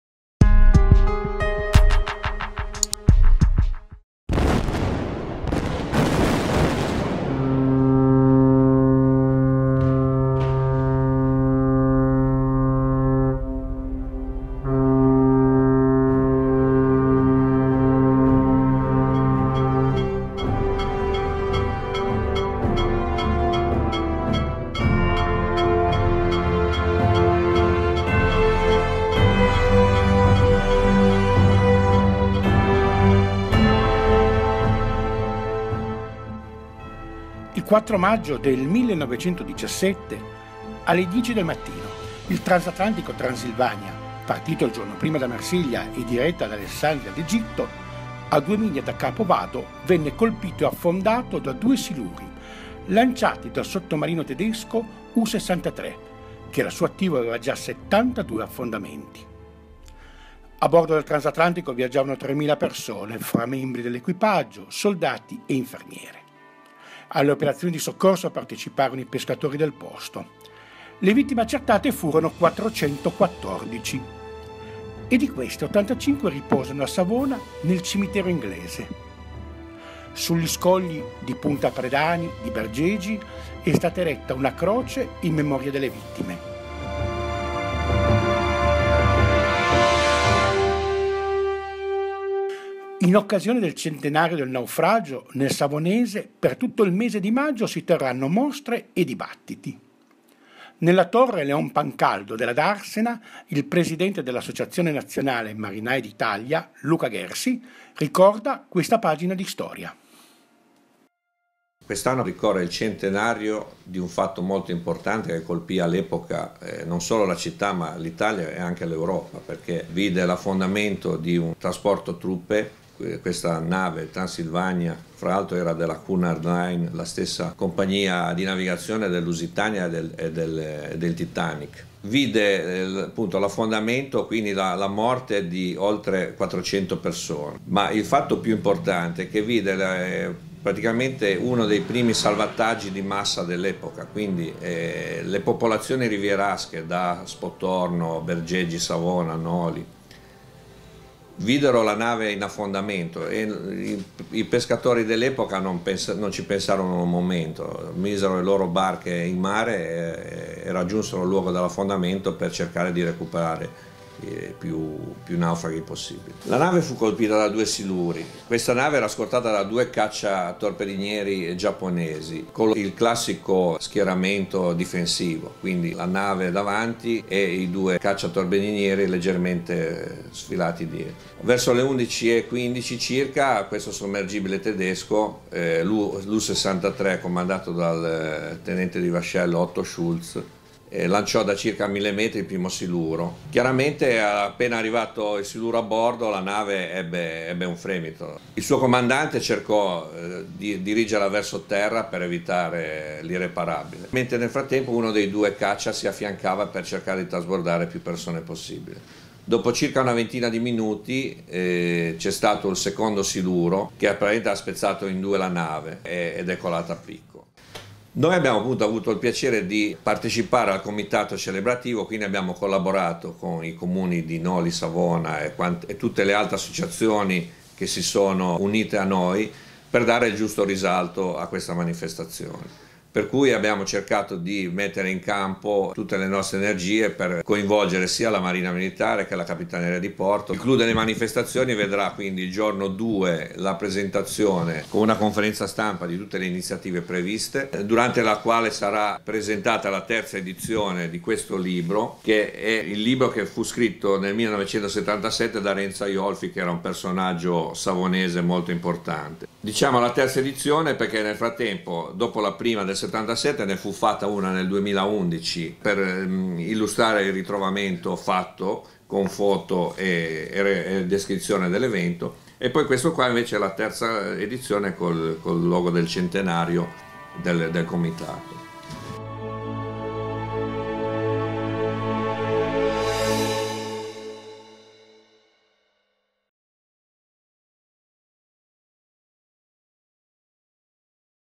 Mostre e dibattiti per non dimenticare l’affondamento del Transylvania
Il 4 maggio del 1917, alle 10 del mattino, il transatlantico Transylvania, partito il giorno prima da Marsiglia e diretto ad Alessandria d’Egitto, a due miglia da capo Vado venne colpito e affondato da due siluri lanciati dal sottomarino tedesco U 63, che al suo attivo aveva già 72 affondamenti. A bordo del transatlantico viaggiavano 3.000 persone fra membri dell’equipaggio, soldati e infermiere. Alle operazioni di soccorso parteciparono i pescatori del posto. Le vittime accertate furono 414 e di queste, 85 riposano a Savona nel cimitero inglese. Sugli scogli di Punta Predani di Bergeggi è stata eretta una croce in memoria delle vittime. In occasione del centenario del naufragio a Savona, per tutto il mese di maggio, si terranno mostre e dibattiti. Nella Torre Leon Pancaldo della Darsena, il presidente dell’associazione Nazionale Marinai d’Italia, Luca Ghersi, ricorda questa pagina di storia.